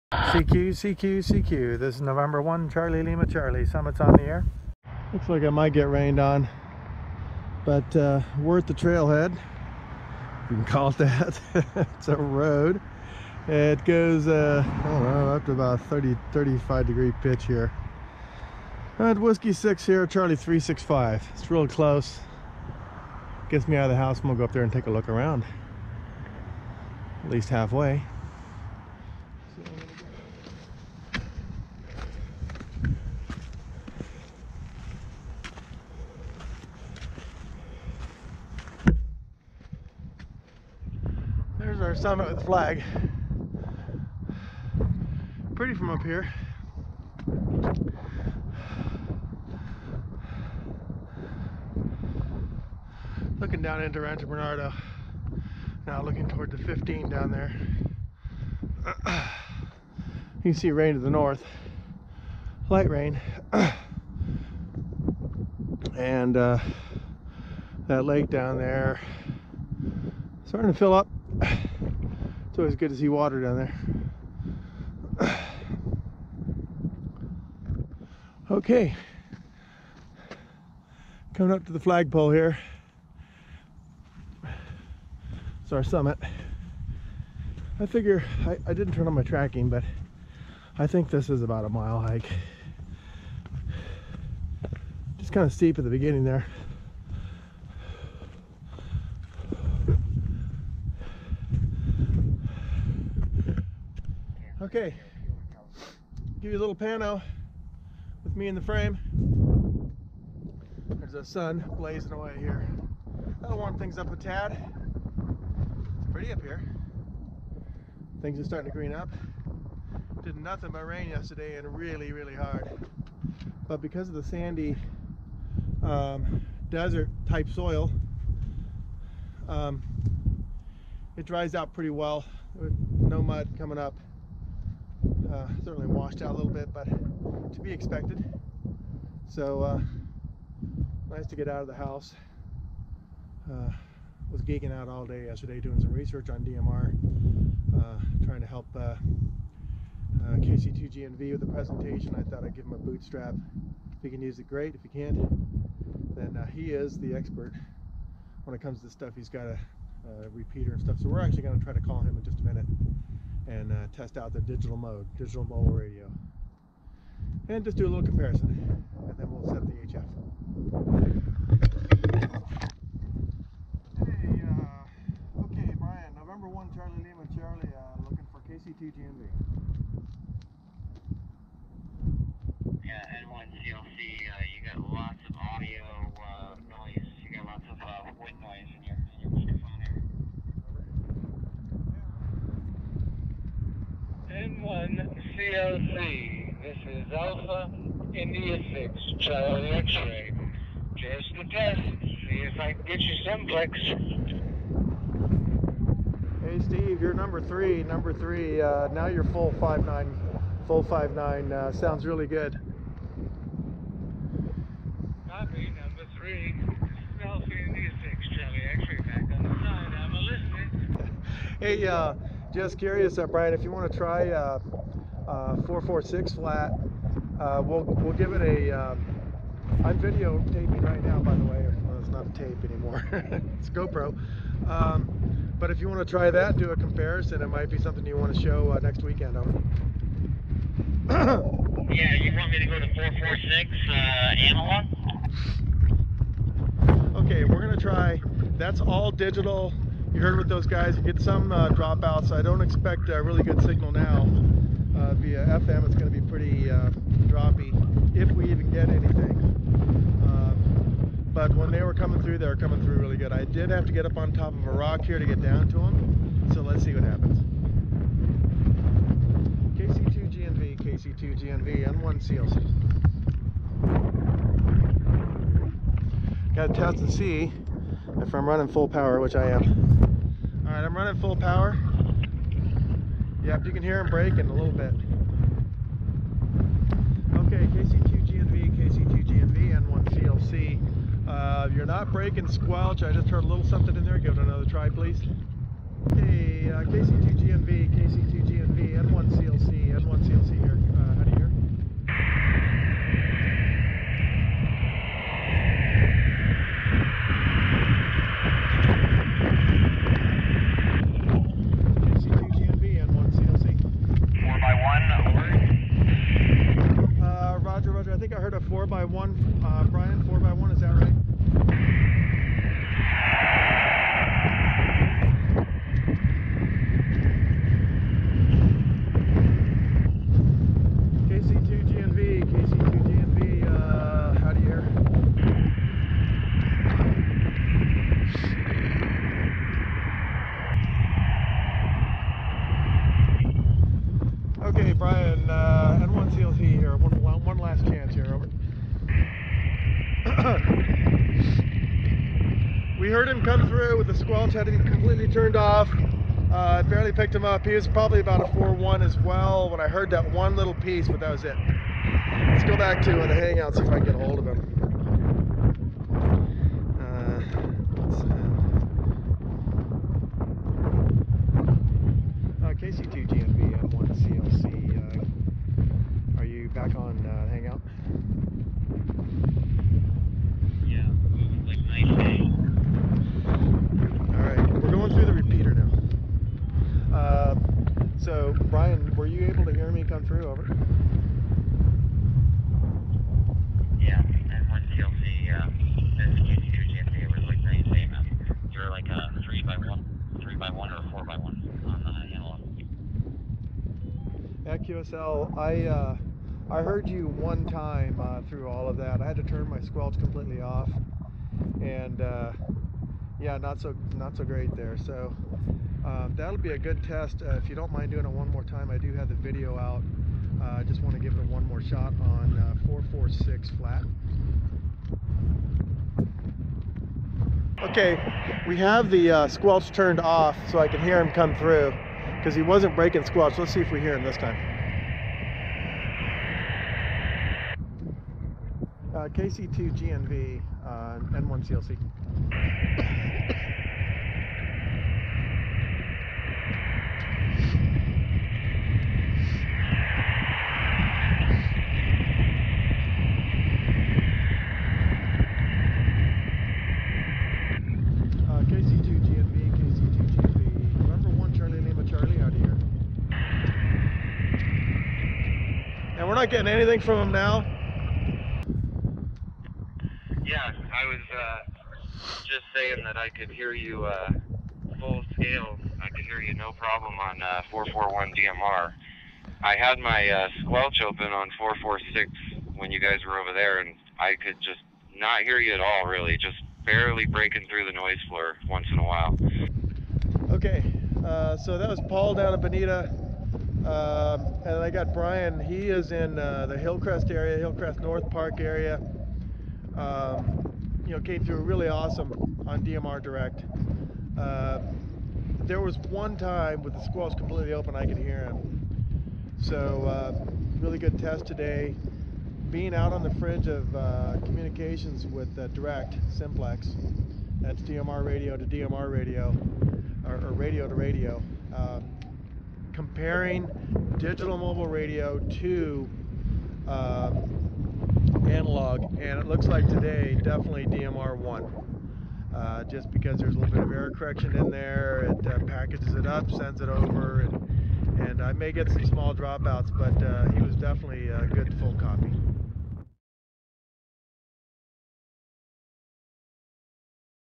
2019-02-03 SOTA and DMR Testing in Rancho Bernardo
Ham Radio and Summits on the air with The Ham Ninja.  I activated a small peak here in San Diego and did a head to head test of digital vs. analog radio.